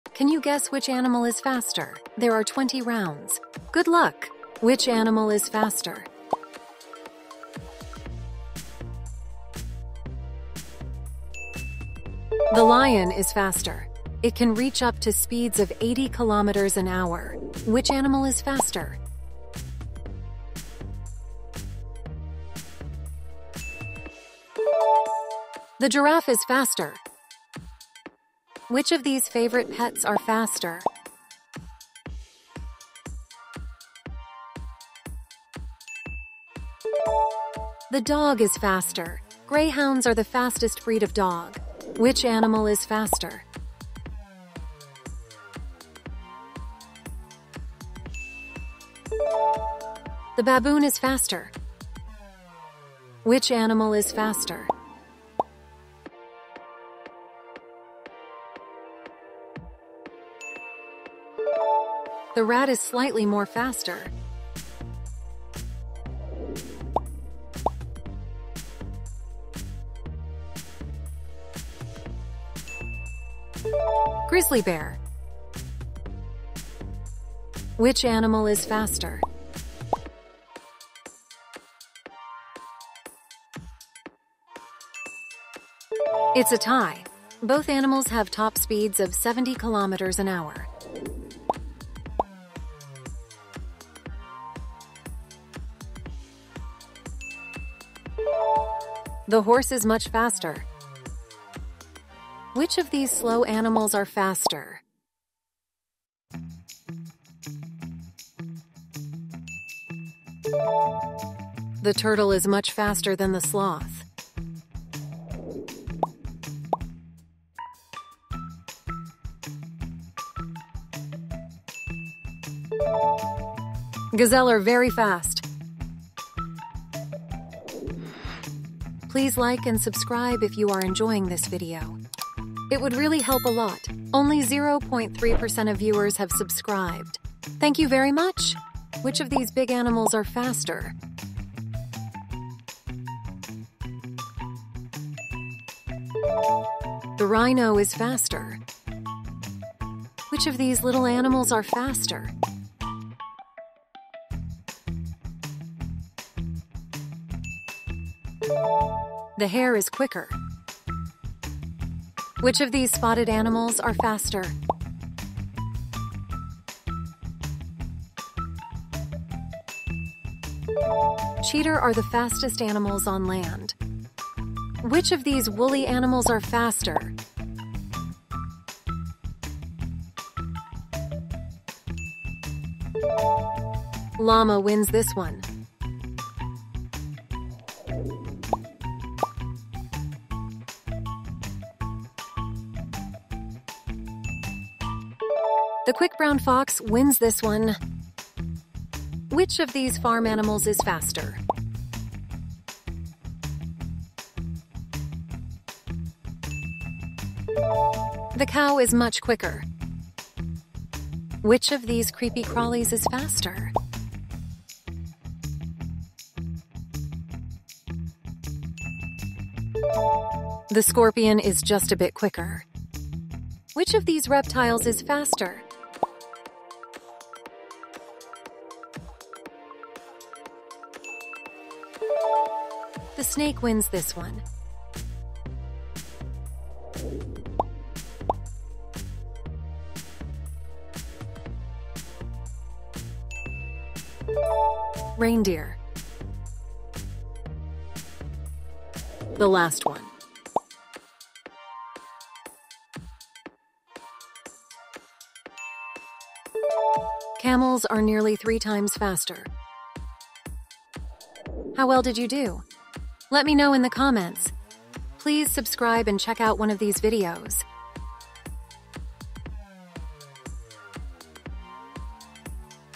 Guess Which Animal Is Faster | Animal Quiz
This is Guess Which Animal Is Faster | Animal Quiz

In this guess the animal challenge, guess which animal is the faster one. Can you guess the fastest animal in time?

In this animal quiz game we will show you 2 animals and your task is to guess which animal is faster. There are total of 20 rounds in this animal puzzle.

Goodluck with the game!